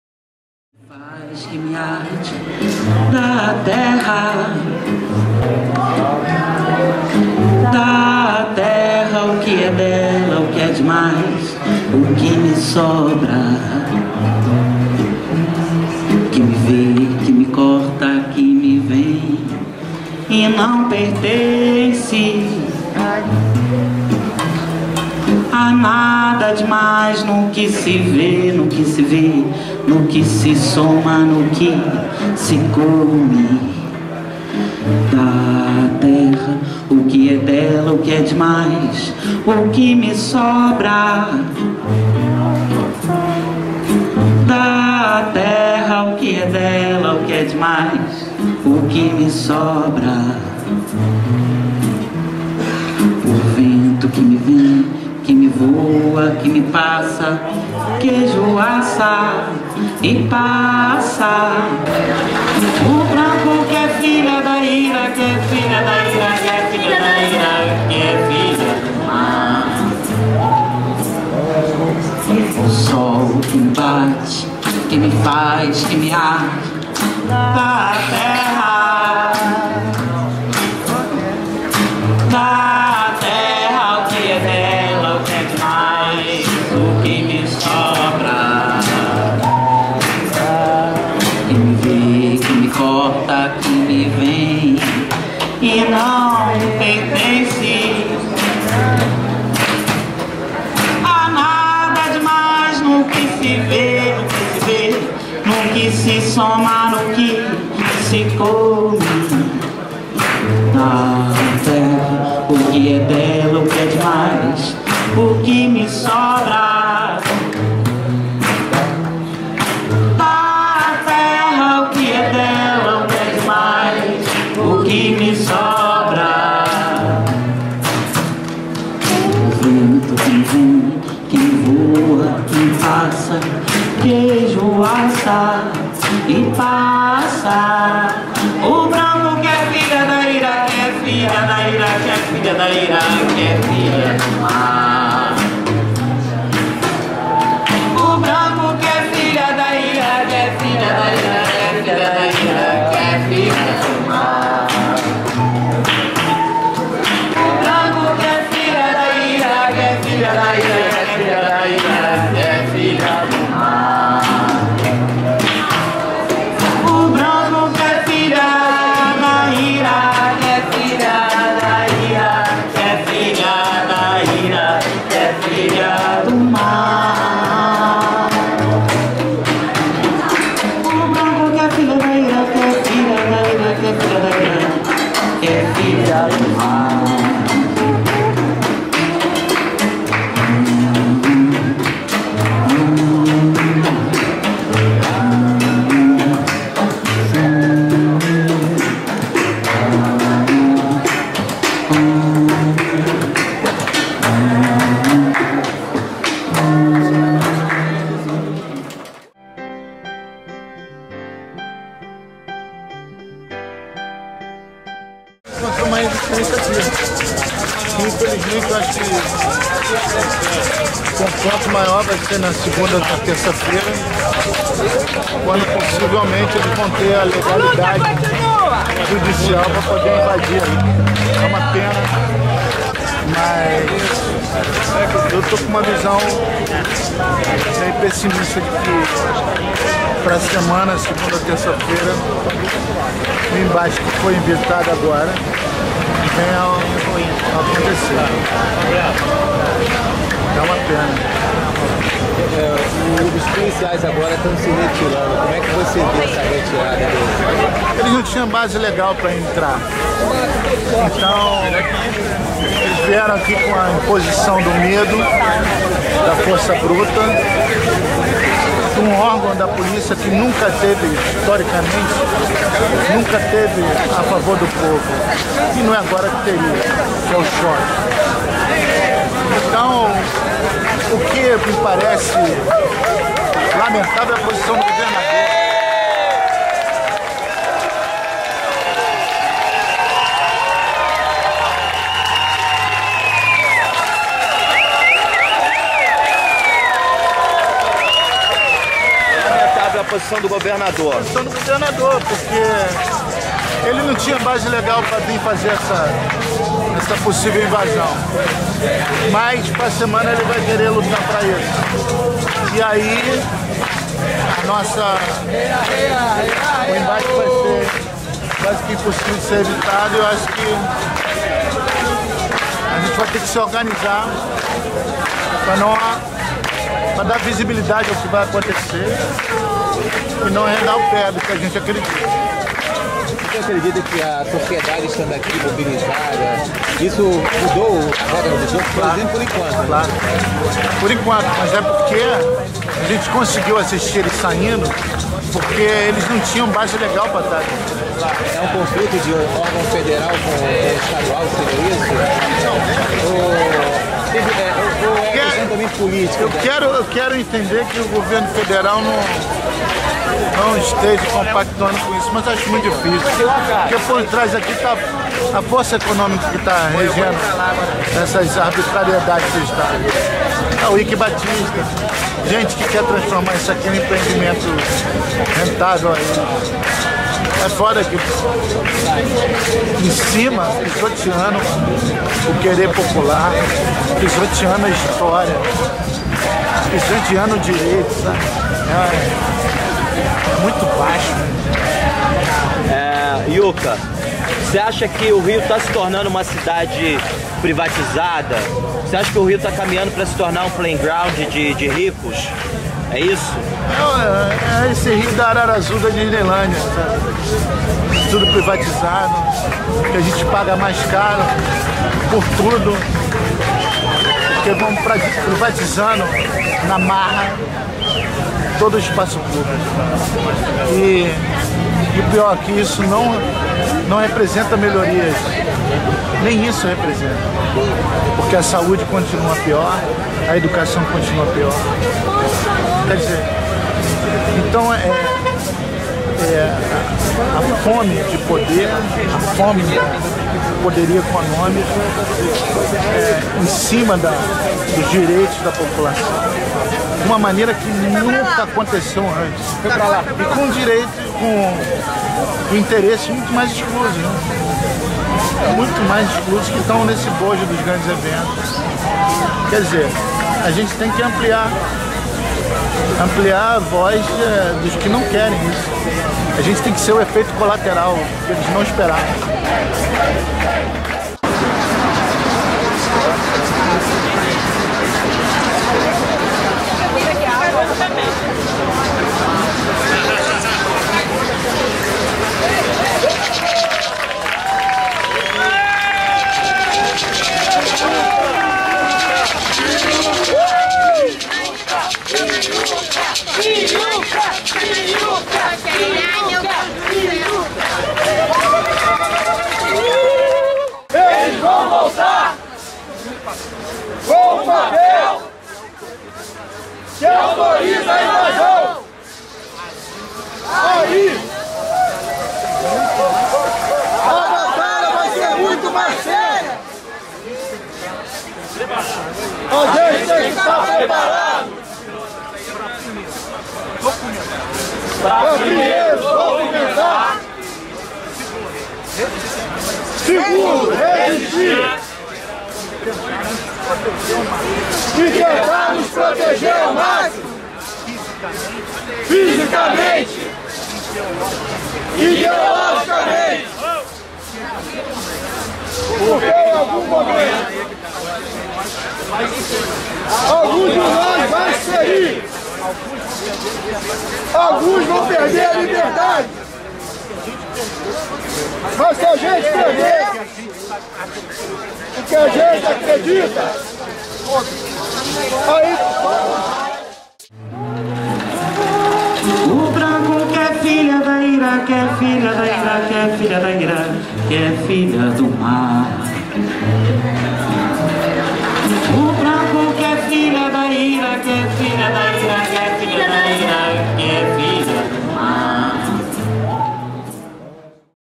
Marcelo Yuka - Herói da Aldeia Maracanã
Herói da resistência da Aldeia Maracanã.
Marcelo Yuka resistiu à Tropa de Choque do Cabral no dia 12/01/2013, no dia da invasão. Parabéns Yuka, você é motivo de orgulho para todos nós!!!